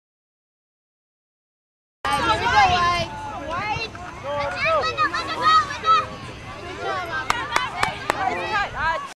Aubrey's Step Over and Cut
Aubrey's performs a step over and then a cut to out manuever her soccer opponent.